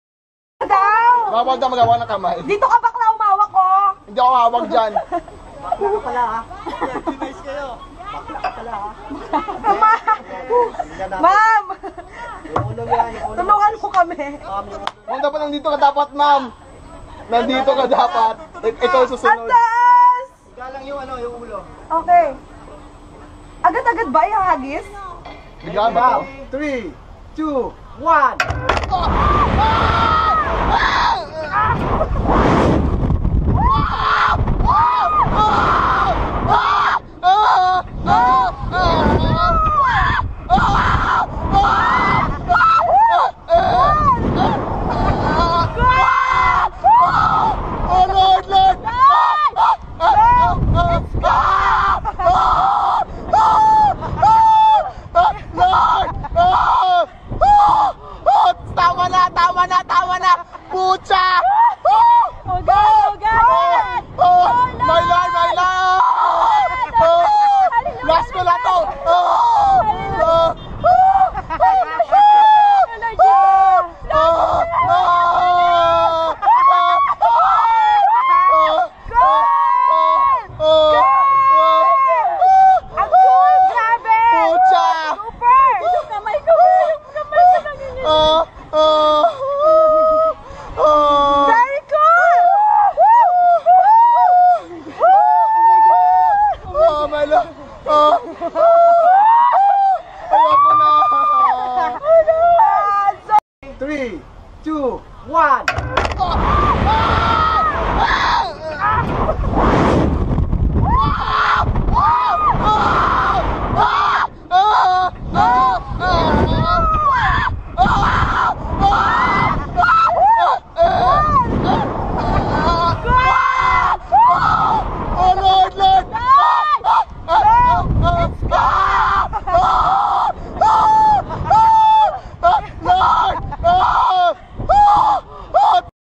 G Max Reverse Bungy Boracay (First Timers and he don't know what to do haha)
Buckle up the safety belts… inhale… and charge skywaaaard!!!

G-Max Reverse Bungee is the latest extreme attraction in Boracay island and it is lifting everyone’s adventurous spirits up since December 2014. Like being the stone in a slingshot and riding a rocket launch, the experience gets adrenaline pumping as one is propelled 50 meters high at a speed of 200kph, allowing gravity to have a little fun. On you.

The ride, operated by Pacific Rim Group of Companies, is originally from New Zealand and is the first of its kind in the Philippines. It has a 100% No Accident Safety Record for over 1 million riders.

MAXimum fun guaranteed. You’ve been warned, though: G-Max works best for the sober and the strong-hearted.

Prepare to skyrocket within the beachfront grounds of Paradise Garden Resort Hotel in Station 3.